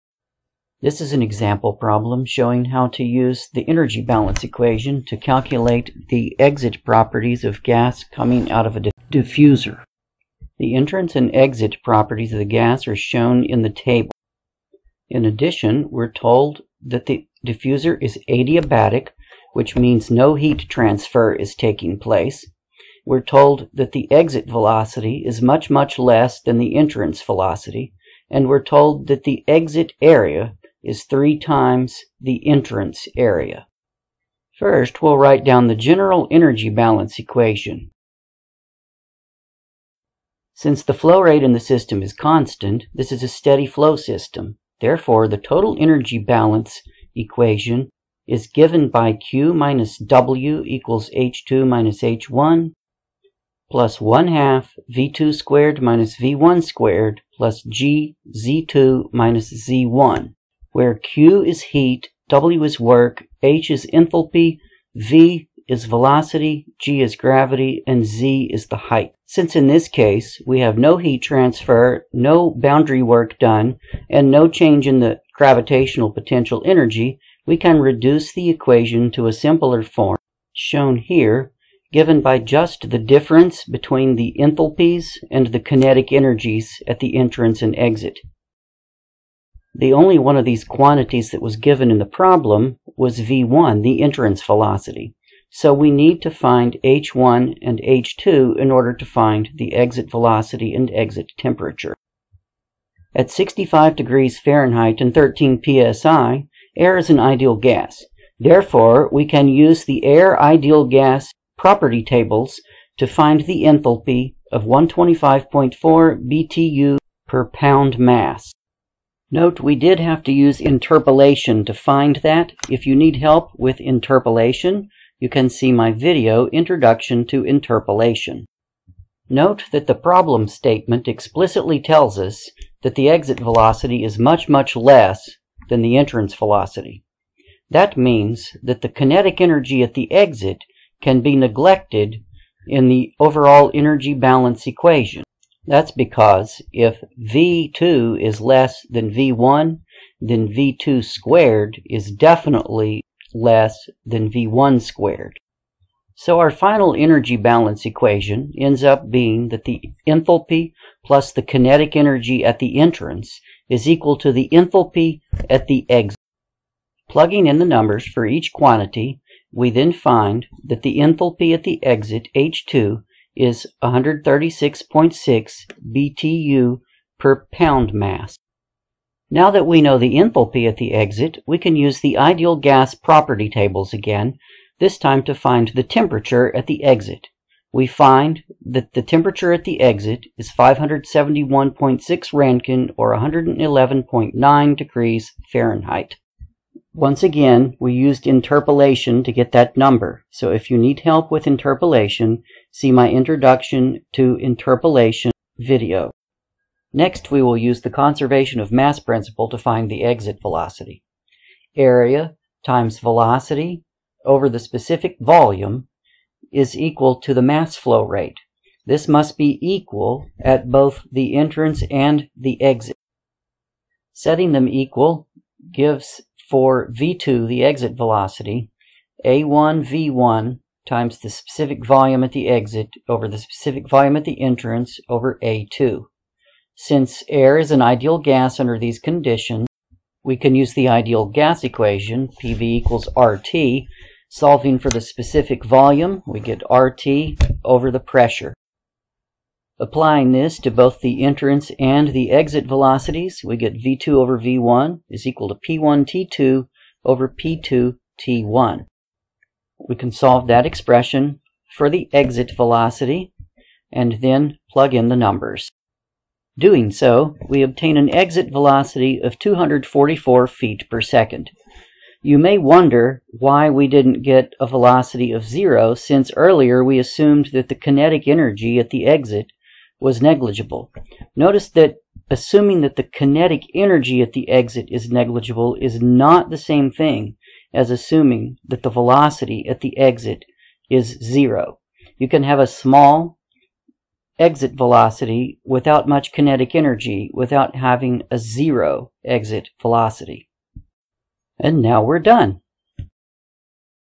Exit velocity and temperature of air from a diffuser
This video shows how to calculate the exit velocity and exit temperature of air that is has passed through a diffuser given the entrance velocity, pressure, and temperature and other conditions using energy balance. The example is based on problem 5-32E from the Cengel and Boles Engineering Thermodynamics text, 8th edition.